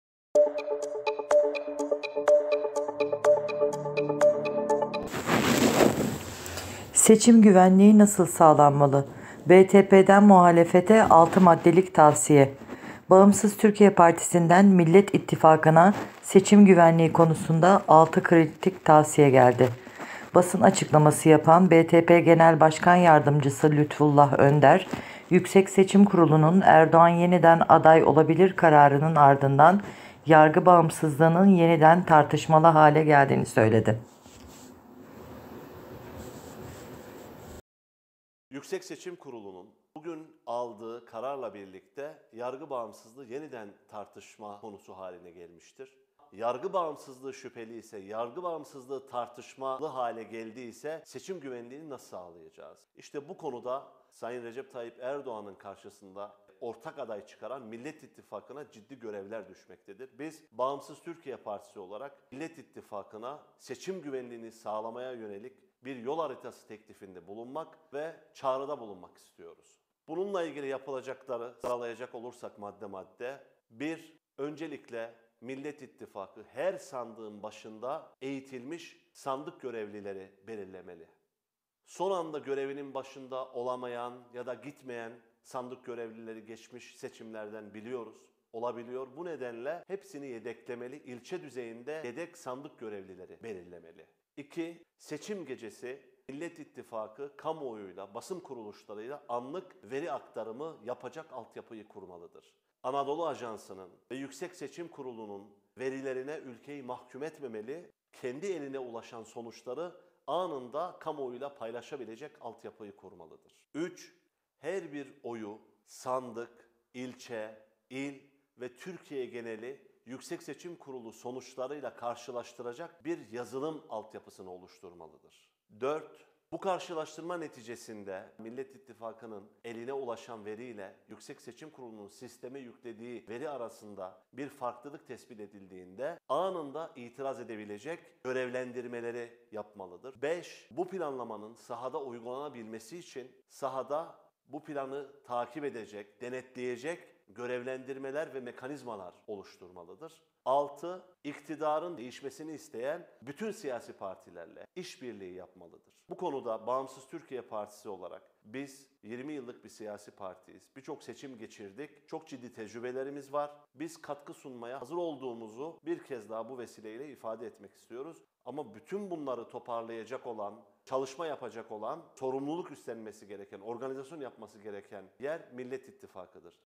Seçim güvenliği nasıl sağlanmalı?
BTP’den muhalefete 6 maddelik tavsiye…

Bağımsız Türkiye Partisi’nden Millet İttifakı’na seçim güvenliği konusunda 6 kritik tavsiye geldi.
Basın açıklaması yapan BTP Genel Başkan Yardımcısı Lütfullah Önder Yüksek Seçim Kurulu’nun Erdoğan yeniden aday olabilir kararının ardından yargı bağımsızlığının yeniden tartışmalı hale geldiğini söyledi.